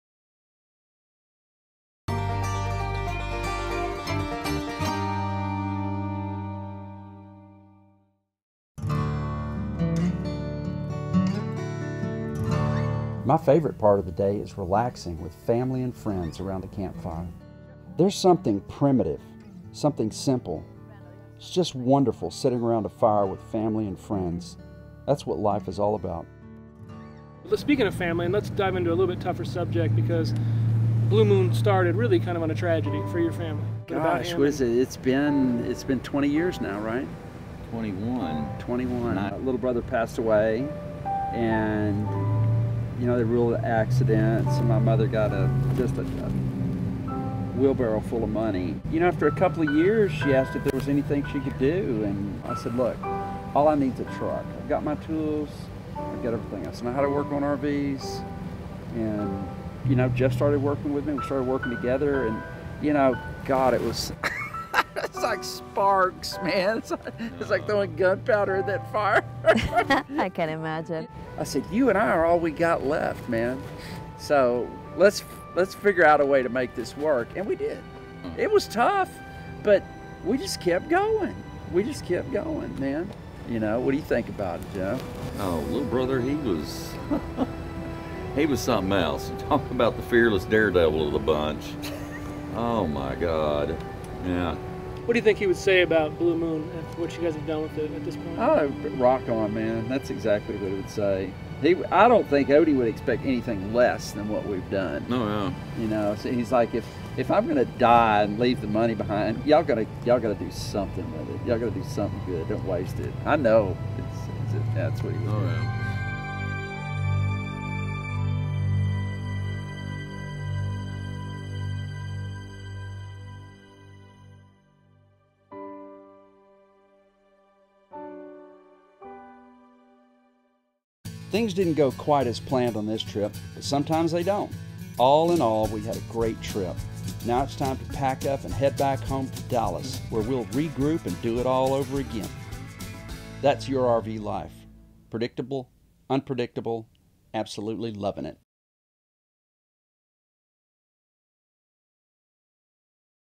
For Odie
Blue Moon was founded soon after a family tragedy for Rodney and Jeff Simmons. The team wraps up a busy day sitting around the campfire reflecting on their baby brother and the early days of Blue Moon.